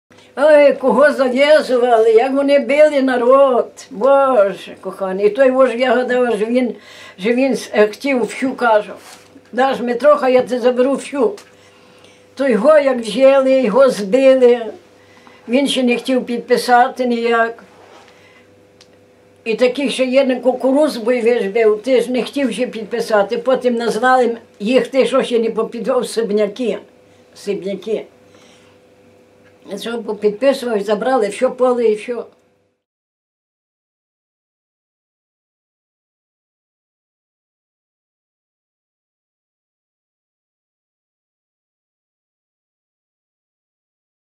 Даниїла Галун про створення колгоспу
Респондент: Даниїла Галун, 1930 р. н.
уродж. с. Куп’ятичі Перемишльського повіту (нині Польща)
мешк. с. Гусаків Мостиського р-ну Львівської обл.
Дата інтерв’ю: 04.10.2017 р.
Інтерв’юери: Юрій Пуківський, Ігор Рочняк